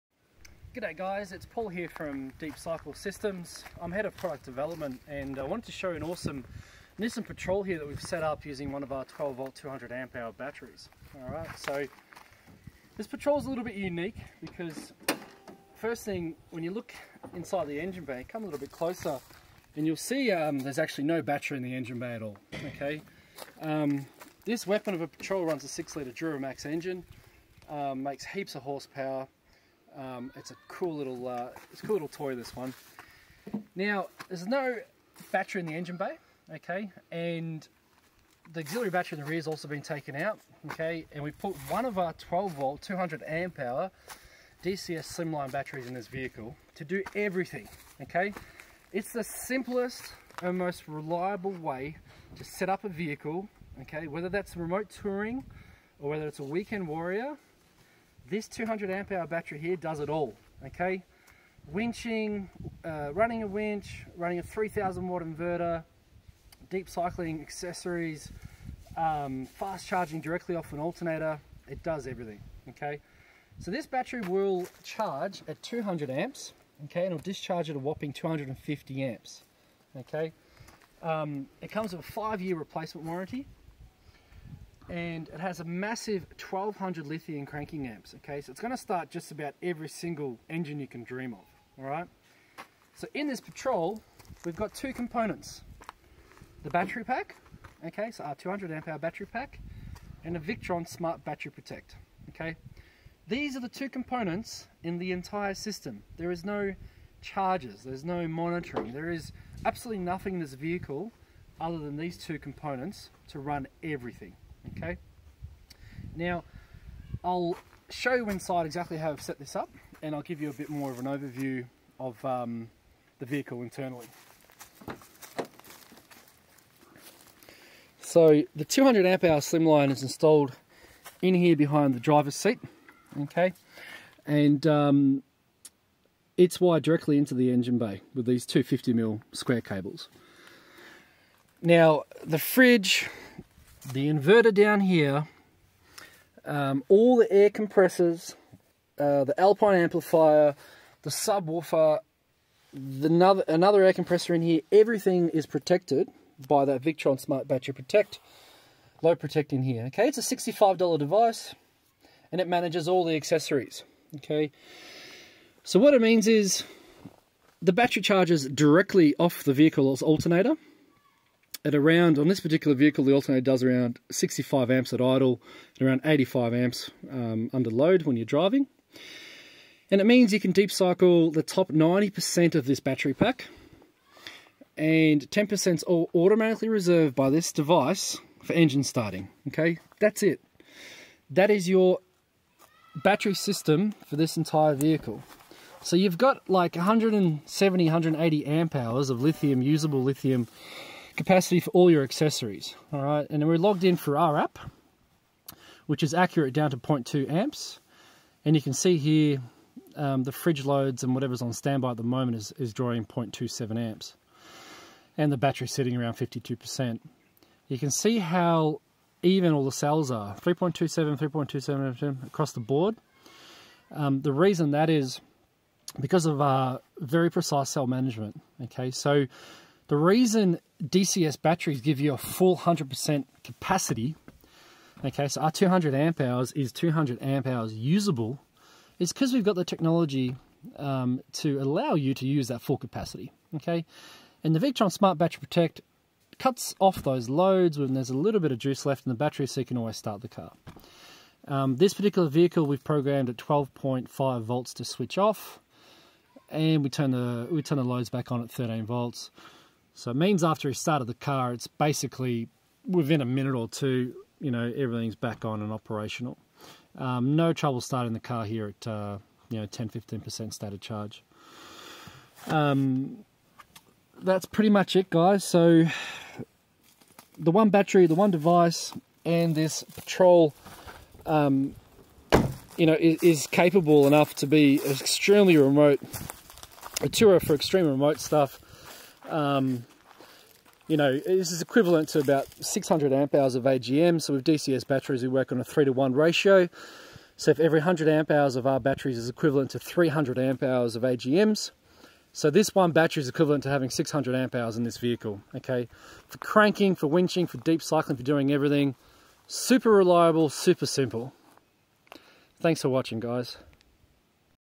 DCS 12V 200Ah Premium Lithium battery powering Patrol Duramax
DCS 200Ah Premium slim line lithium high performance batteries

Paul from Deep Cycle Systems showcases an impressive Nissan Patrol equipped with their 12V 200Ah Slimline Lithium Battery, replacing both the engine bay and auxiliary batteries. This powerful setup runs a 6L Duramax engine and handles everything from winching to powering a 3000W inverter, all while charging directly from the alternator. The system includes just two components—the DCS battery and a Victron Smart Battery Protect—ensuring reliability for remote touring or weekend adventures. With a 5-year warranty, 1200A cranking power, and smart load management, this setup delivers 180Ah of usable lithium capacity while keeping the vehicle always ready to start.

Unit 2, 5 McPhail Road
Coomera, QLD 4209
Australia